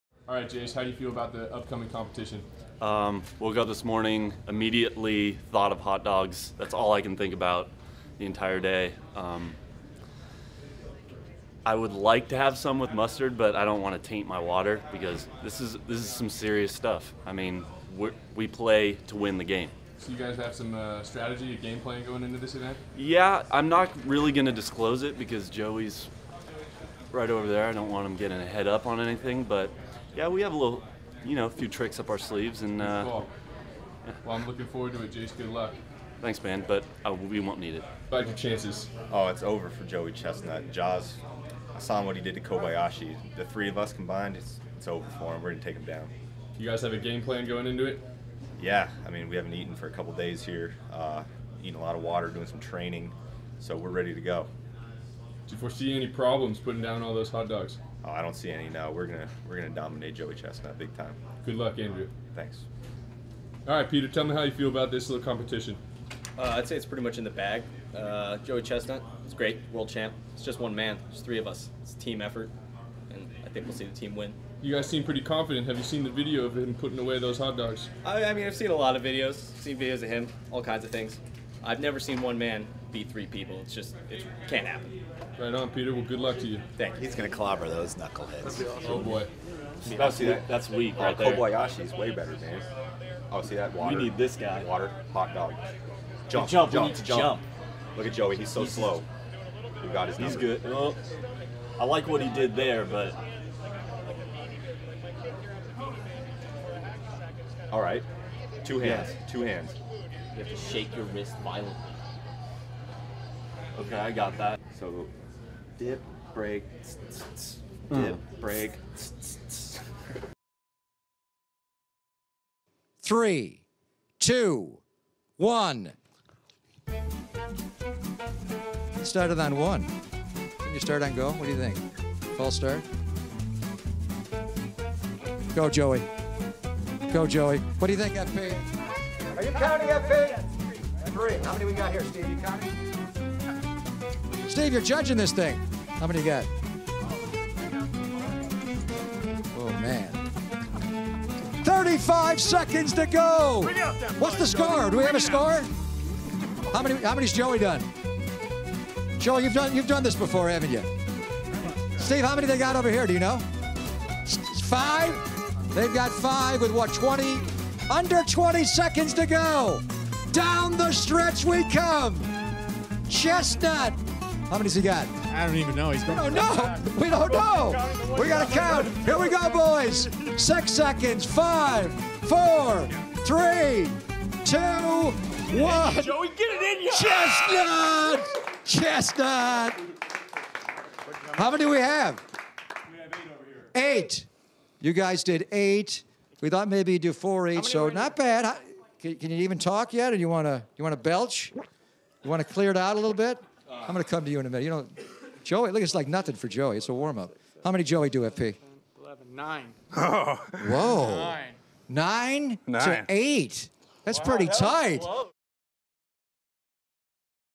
Joey Chestnut takes on 3 employees in a Hot Dog eating competition
Joey Chestnut took on three CSN Production Assistants in a 60 second hot dog eat-off. Complete with pregame interviews, see how he fared.